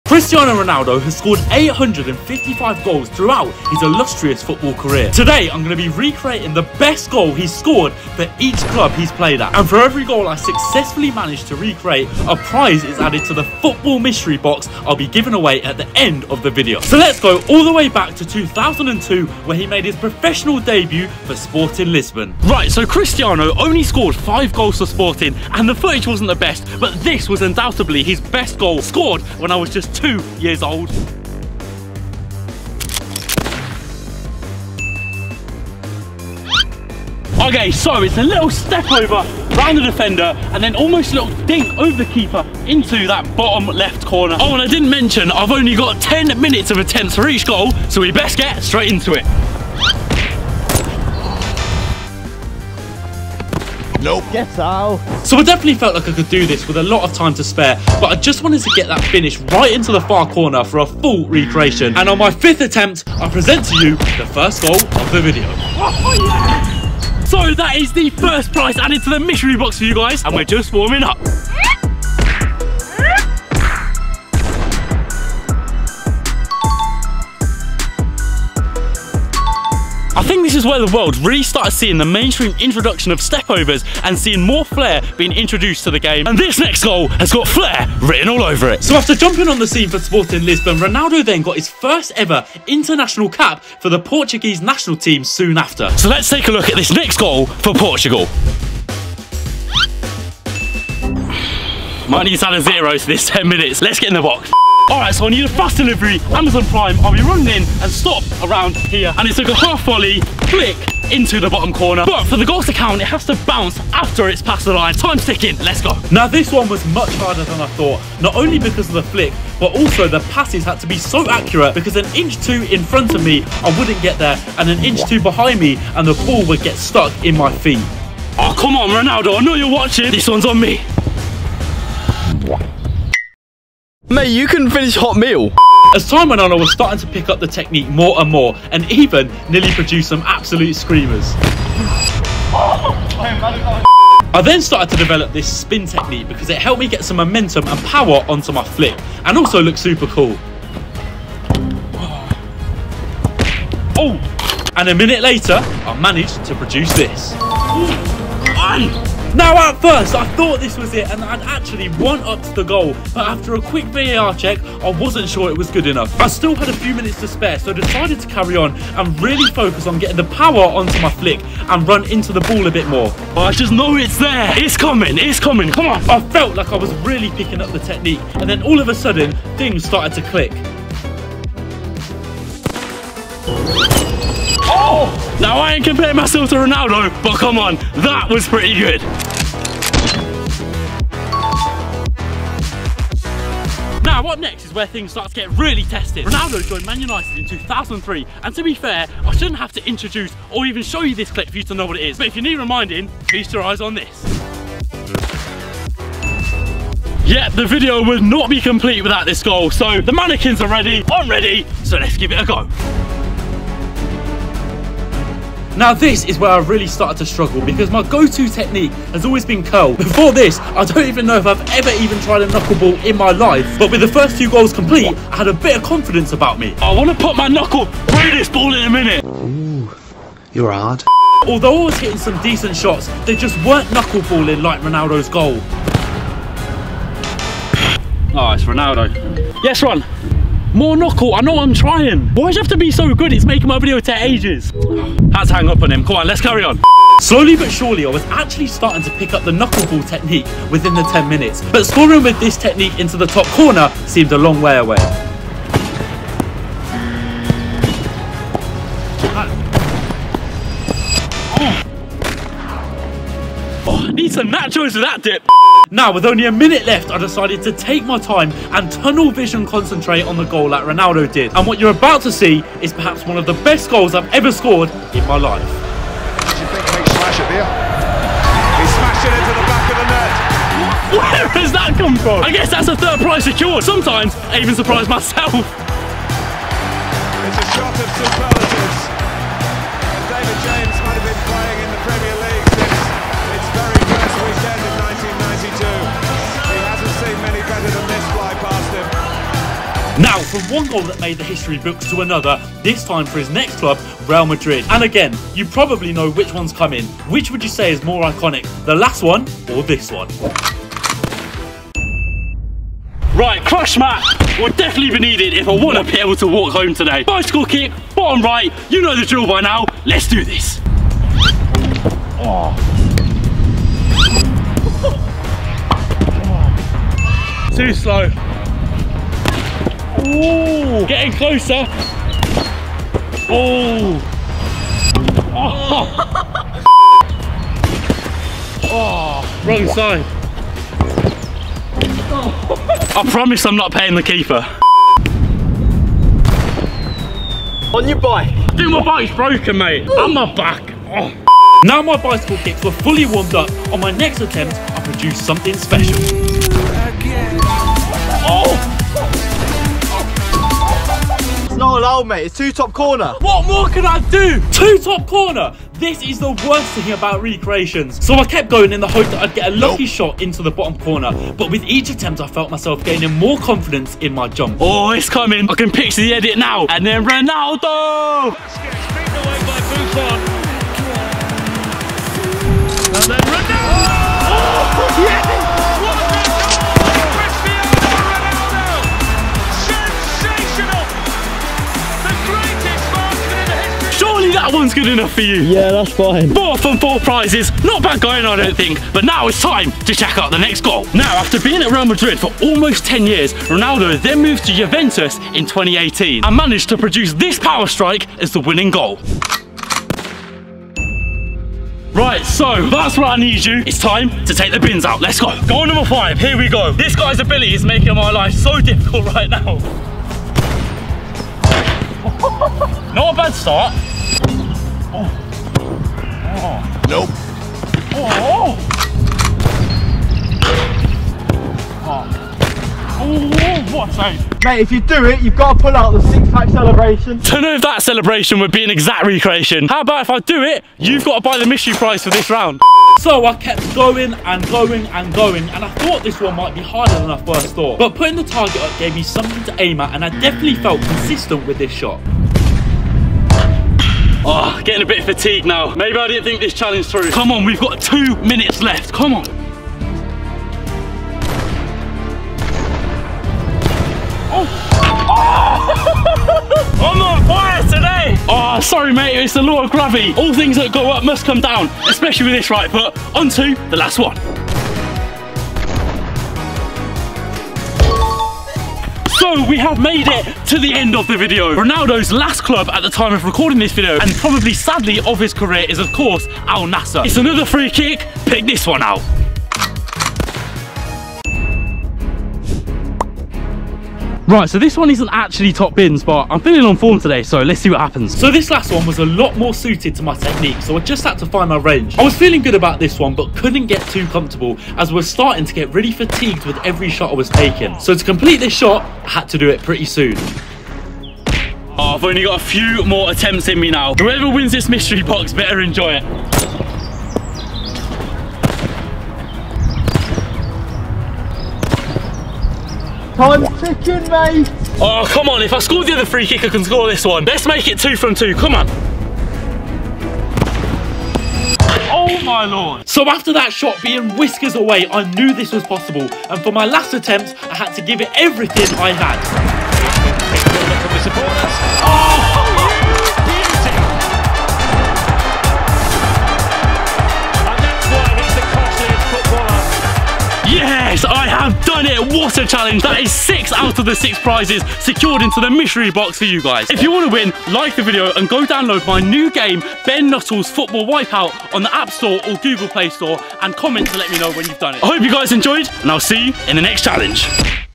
I Recreated Ronaldo’s Best Goal At Every Club
In today's challenge, I have an hour to recreate Cristiano Ronaldo's best goal for every club he's played at! ⚽️🎯🔥 Every goal = 1 prize secured for you guys! 🤩